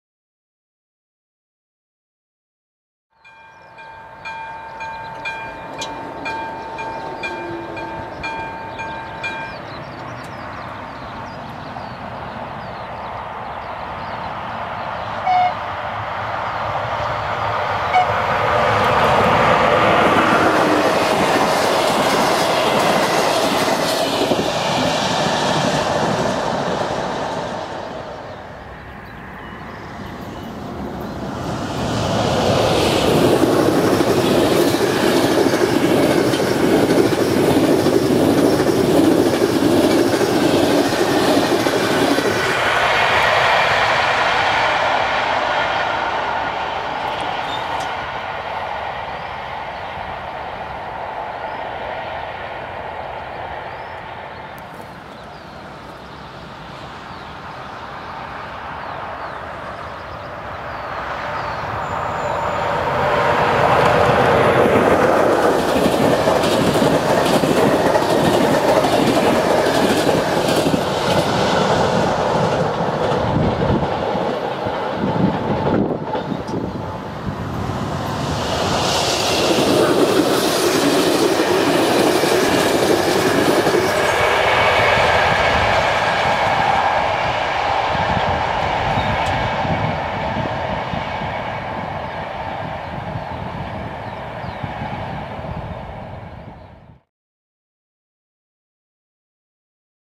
Treni passeggeri a Poprad - Passenger trains in Poprad
Treni passeggeri in transito presso la città di Poprad, in Slovacchia.

Passenger trains running near the city of Poprad, in Slovakia.